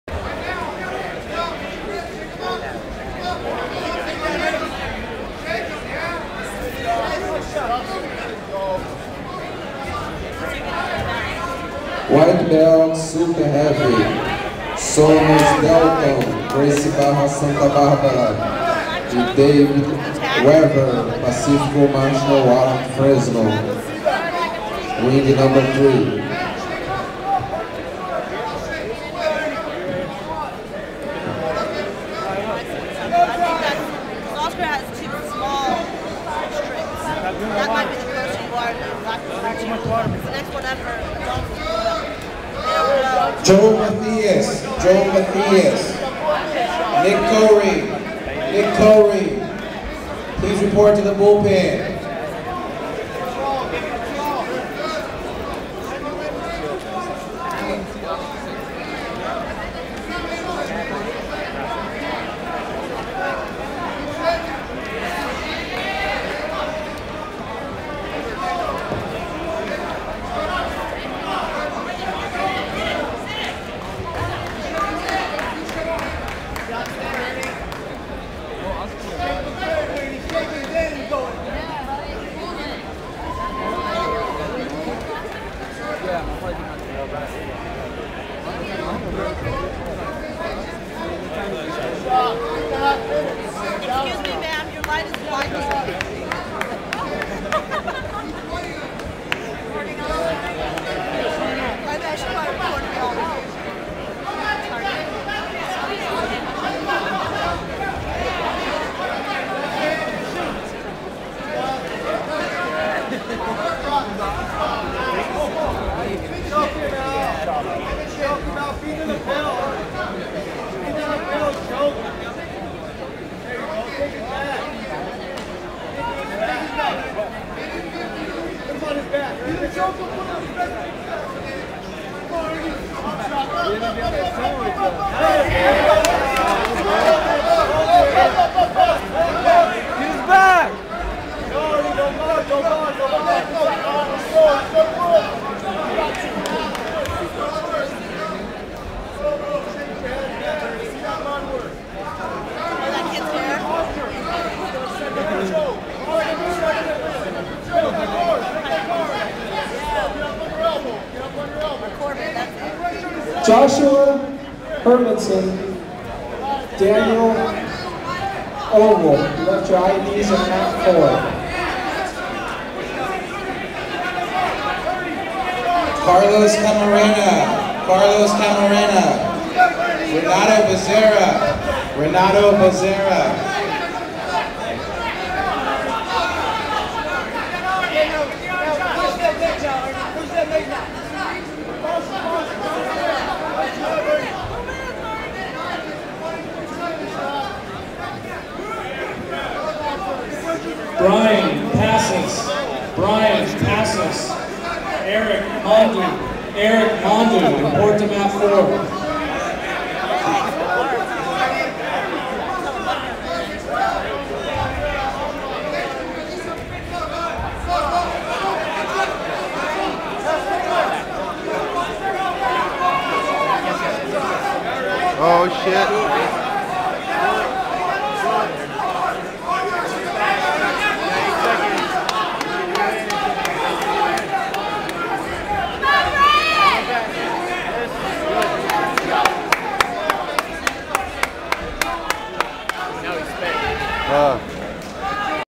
BJJ US Open XIII - Jason Smith - Claudio Franca San Jose
Jason takes on some big-ass-dude from some place